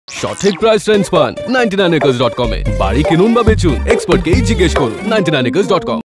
PriceTrends | Get sahi price trends on 99acres.com – Bengali – 6 Sec
Confused about the right price of a property? Check price trends of different properties and make the right property decision with 99acres.com!